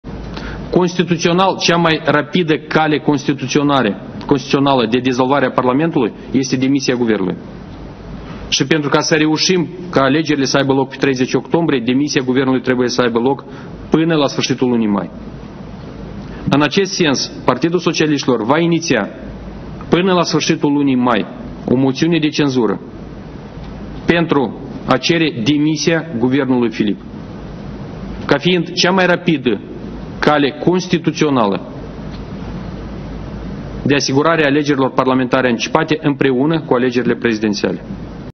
Conferință de presă susținută de președintele PSRM, Igor Dodon, la Strasbourg
PSRM va iniția o moțiune de cenzură pentru demiterea guvernului Filip
Titlul evenimentului: Conferință de presă susținută de președintele PSRM, Igor Dodon, la Strasbourg